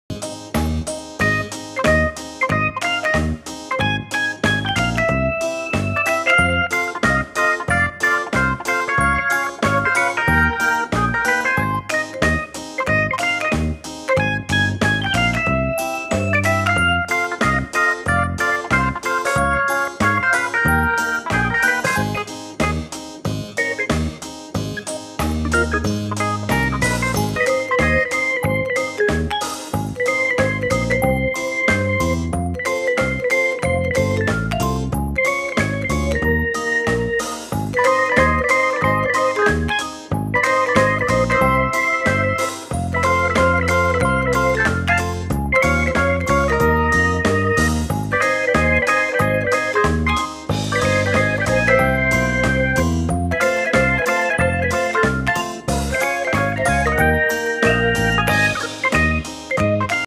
coloring a deer🦌, coloring page for kids and toddlers
Hi guys,welcome to "DRAW WITH MOZA",let's learn how to coloring a deer together with me!